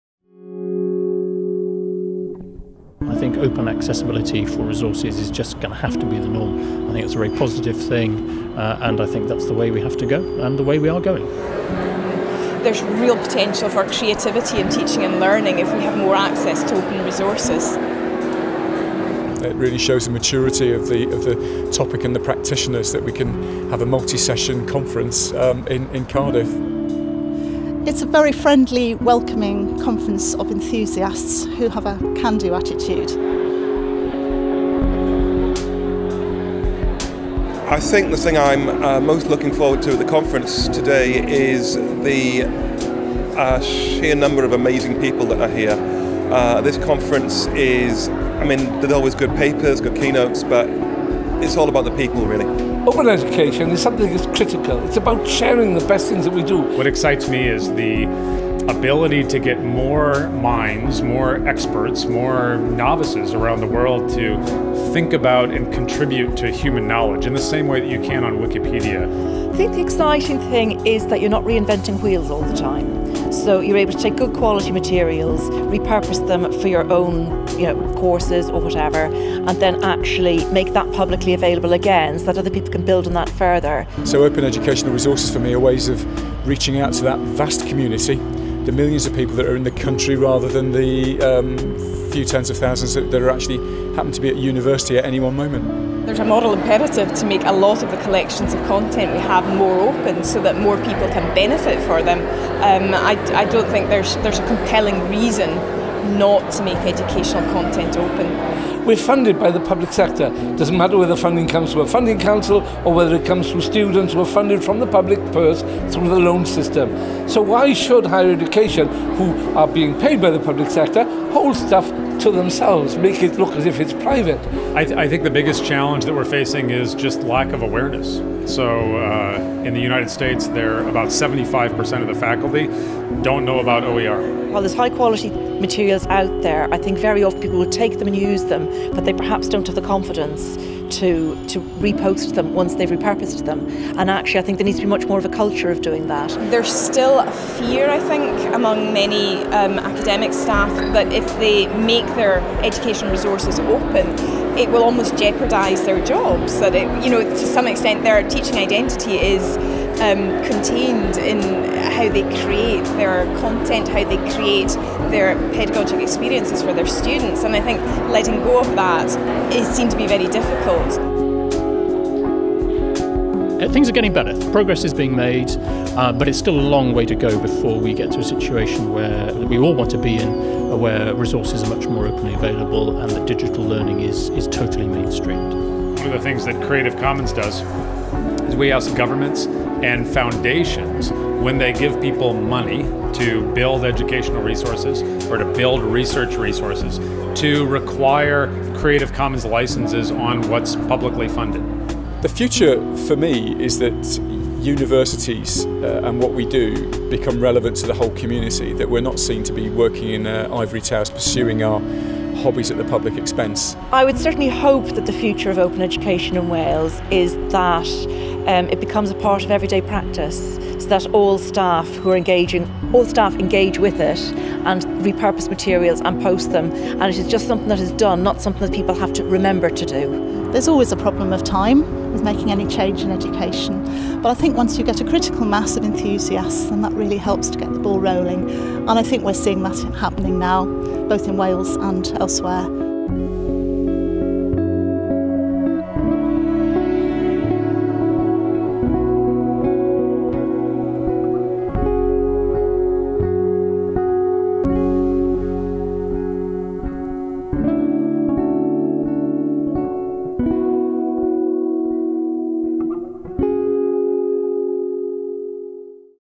The Voice of OER15
A collection of interviews with attendees at the OER15 conference in Cardiff, Wales. They highlight the importance of open education and their hopes for the future. Discover fabulous resources about how to create media in education and examples from across Welsh Universities